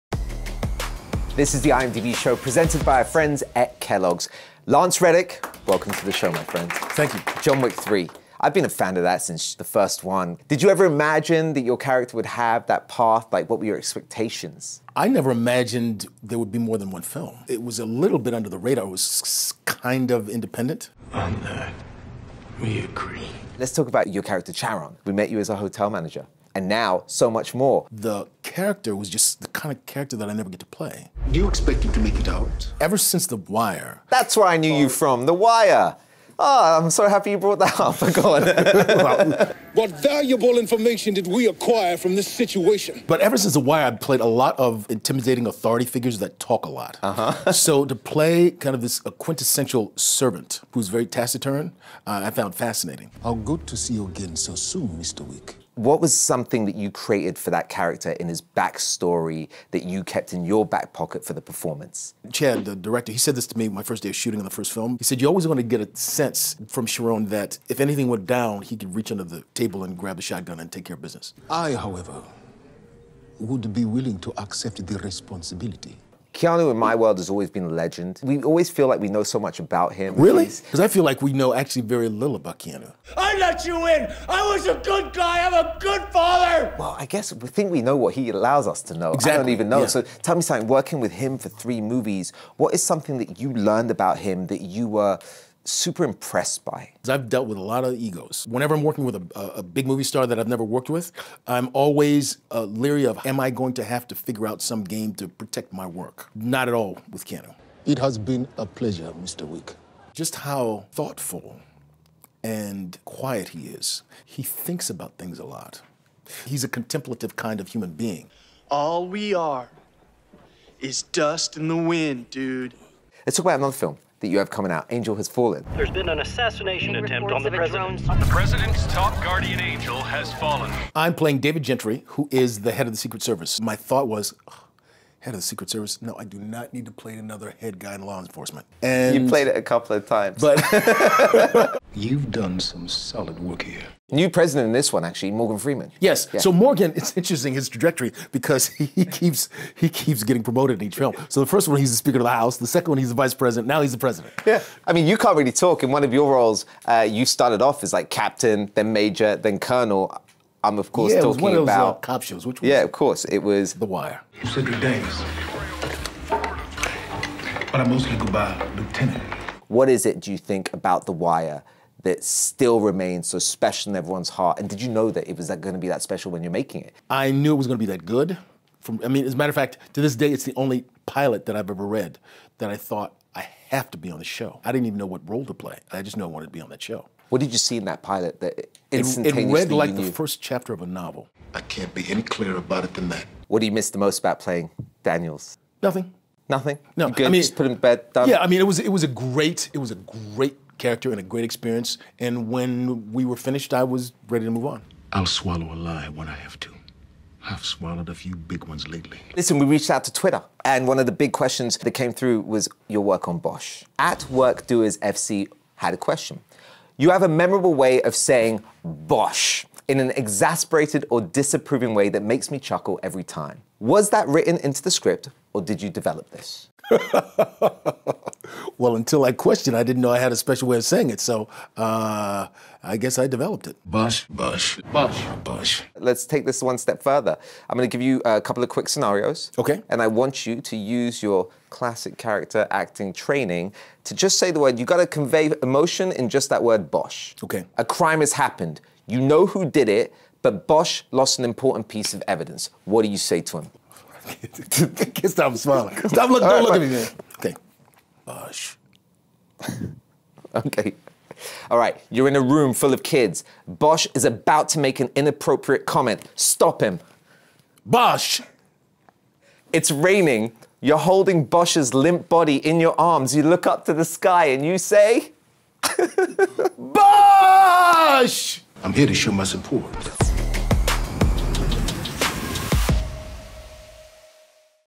Lance Reddick Shows a New Side in 'John Wick' and Says "Bosch" His Own Way
The acclaimed actor stretches his range with 'John Wick' and shares how he says the name of a certain L.A.P.D. detective.